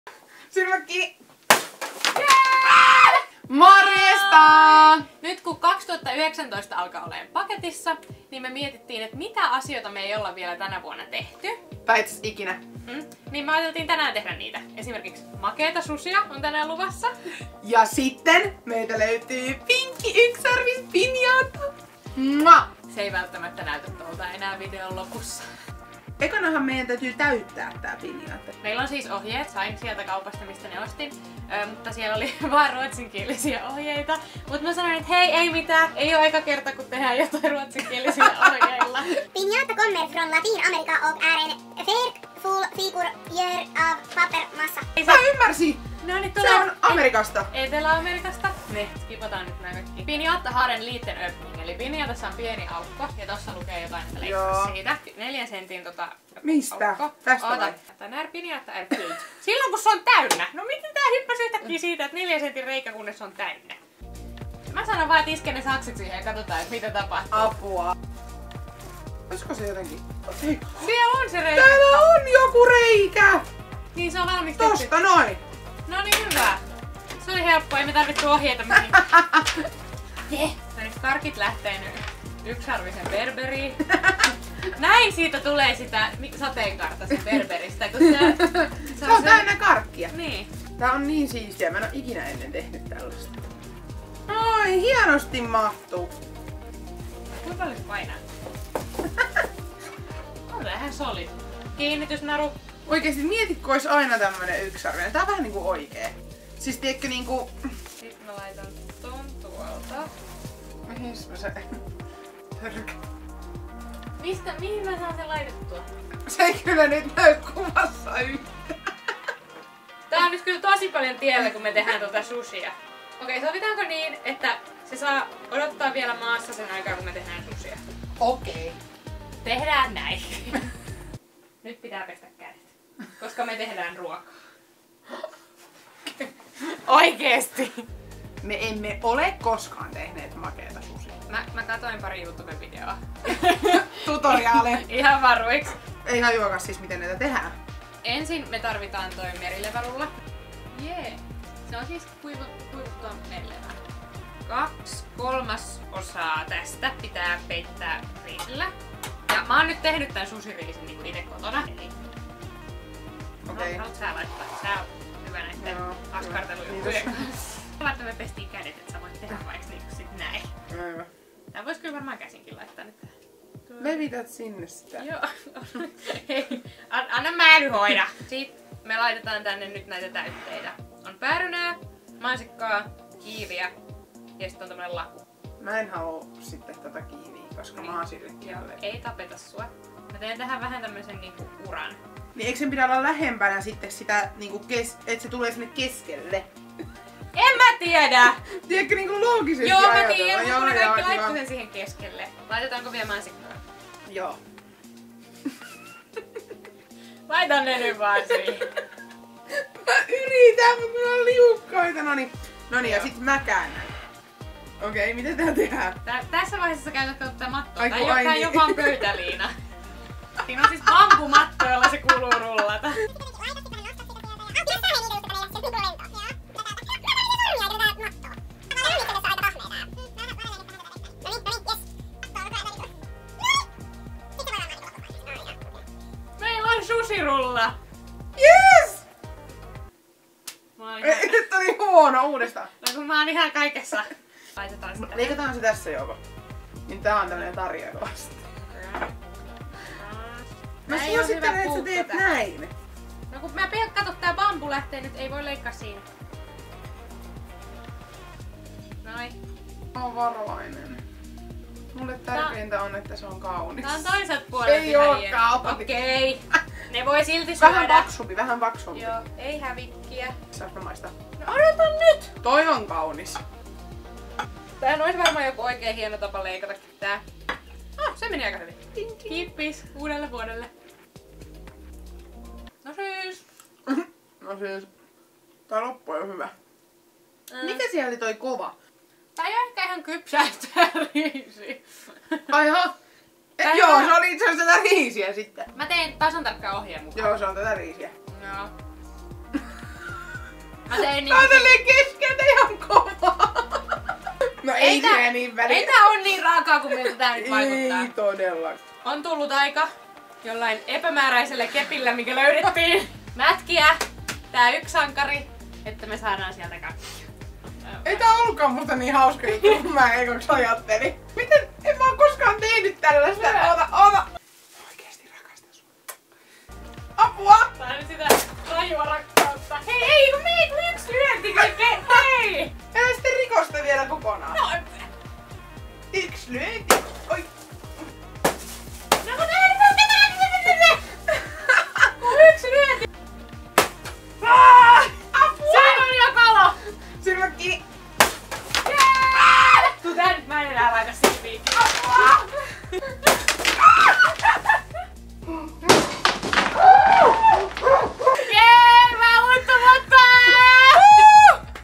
UUSI VUOSI 2020 -HAASTE
Kokeillaan videolla juttuja uuden vuoden kunniaksi, joita ei olla tehty koskaan aiemmin! Saadaanko hajotettua Pinjata? Tuleeko makeasta sushista maukasta?

🤖Sovellus: Galaxi (Google Play & App Store)
📺TV2: Joka aamu!
🎮Netti: yle.fi/galaxi